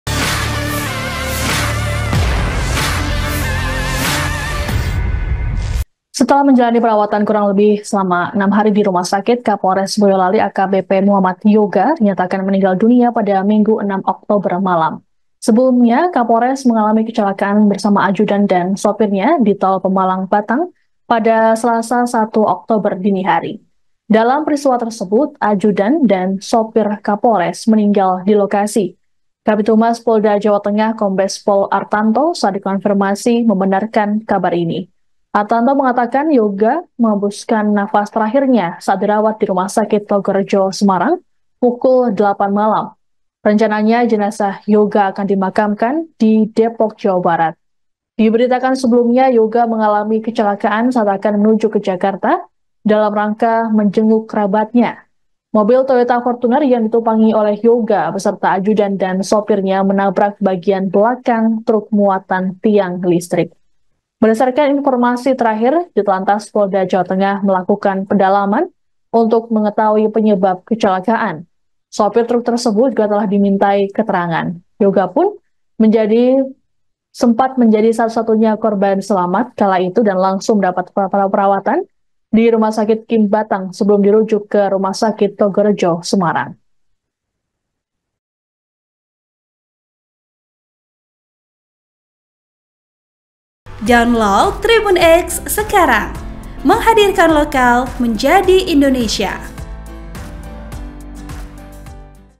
Kapolres Boyolali Meninggal Dunia, Sempat Mendapat Perawatan di Rumah Sakit seusai Kecelakaan
TRIBUN-TIMUR.COM - Setelah menjalani perawatan kurang lebih enam hari di rumah sakit, Kapolres Boyolali AKBP Muhammad Yoga dinyatakan meninggal dunia pada Minggu (6/10) malam.

Sebelumnya Kapolres mengalami kecelakaan bersama ajudan dan sopirnya di Tol Pemalang-Batang pada Selasa (1/10) dini hari.

Dalam peristiwa tersebut, ajudan dan sopir kapolres meninggal di lokasi.

Kabid Humas Polda Jawa Tengah Kombes Pol Artanto saat dikonfirmasi membenarkan kabar ini.

Artanto mengatakan, Yoga menghembuskan nafas terakhirnya saat dirawat di Rumah Sakit Tlogorejo Semarang pukul 20.00 WIB.

Rencananya jenazah Yoga akan dimakamkan di Depok, Jawa Barat.

Diberitakan sebelumnya, Yoga mengalami kecelakaan saat akan menuju ke Jakarta dalam rangka menjenguk kerabatnya.

Mobil Toyota Fortuner yang ditumpangi Yoga beserta ajudan dan sopirnya menabrak bagian belakang truk muatan tiang listrik.

Berdasarkan informasi terakhir, Ditlantas Polda Jawa Tengah melakukan pendalaman untuk mengetahui penyebab kecelakaan.

Sopir truk tersebut juga telah dimintai keterangan.

Yoga pun menjadi satu-satunya korban selamat kala itu dan langsung mendapat perawatan di Rumah Sakit QIM Batang sebelum dirujuk ke RS Tlogorejo Semarang.

(TRIBUN-TIMUR.COM)